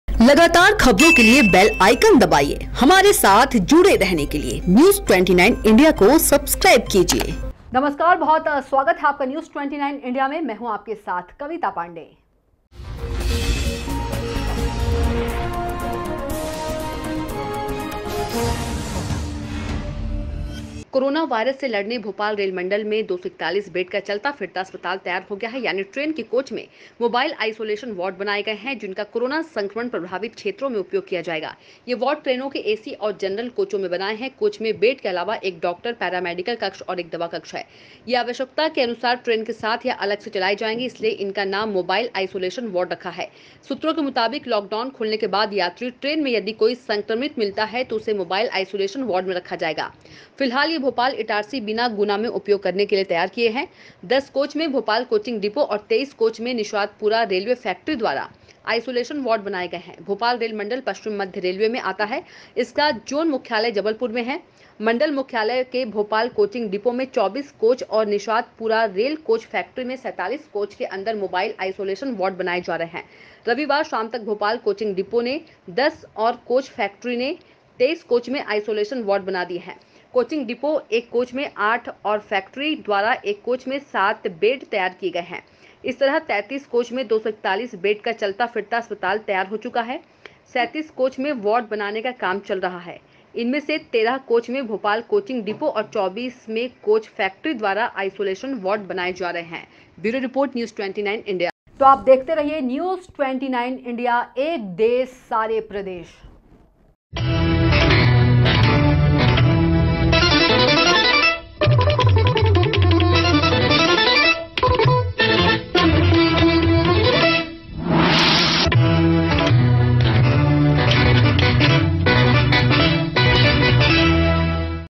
07_April News29India# News 32#कोविड-19 से जंग के लिए पश्चिम रेलवे ने तैयार किया चलता फिरता अस्पताल
कोविड-19 से जंग के लिए पश्चिम रेलवे ने तैयार किया चलता फिरता अस्पताल
भोपाल रेल मंडल में 241 बेड का चलता-फिरता अस्पताल तैयार